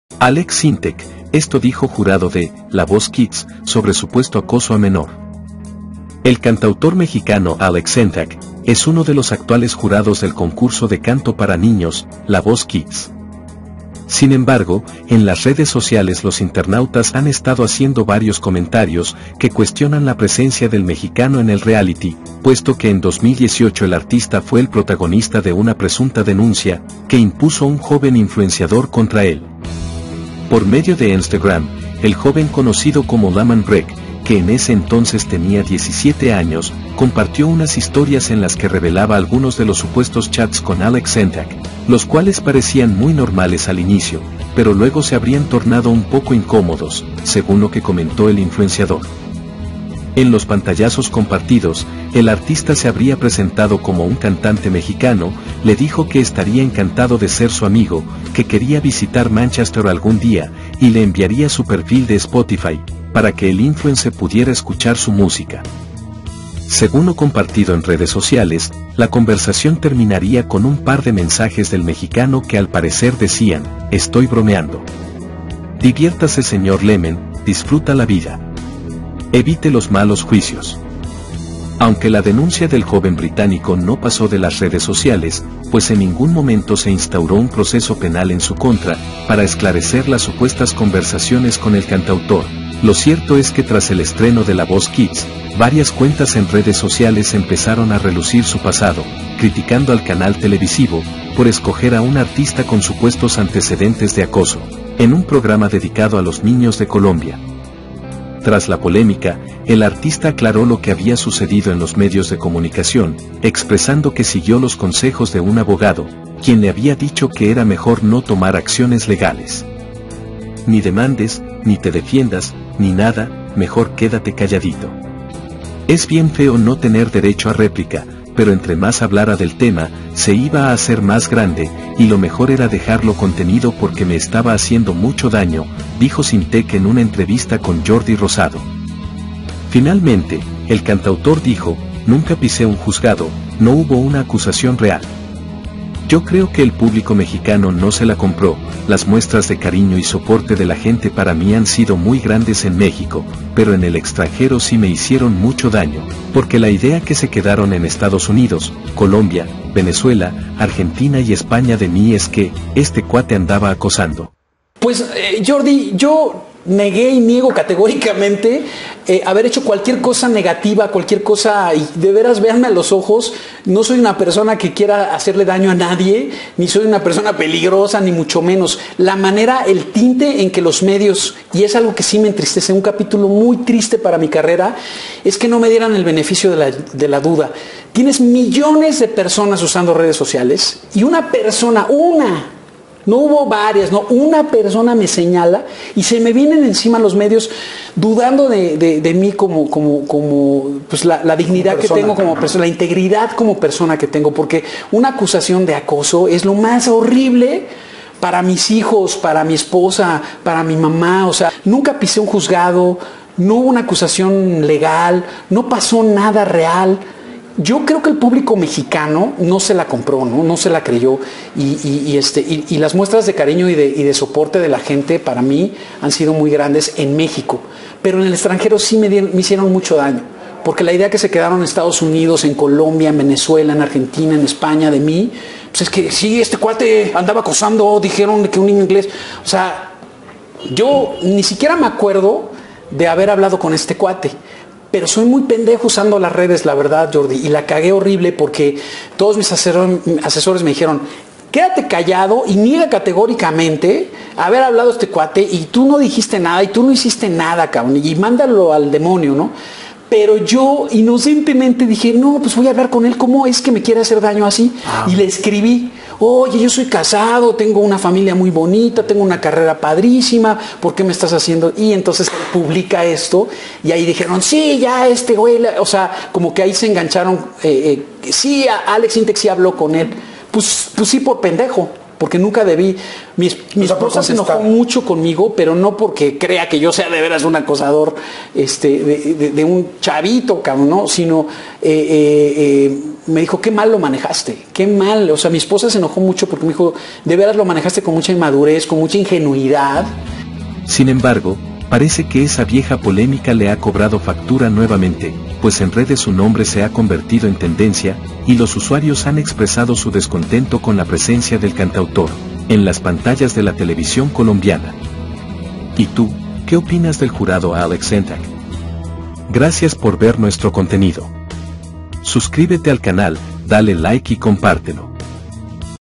💥Acaba de pasar! JURADO DE "LA VOZ KIDS"! Fuerte escándalo!
En redes su nombre se ha convertido en tendencia, y los usuarios han expresado su descontento con la presencia del cantautor, en las pantallas de la televisión colombiana.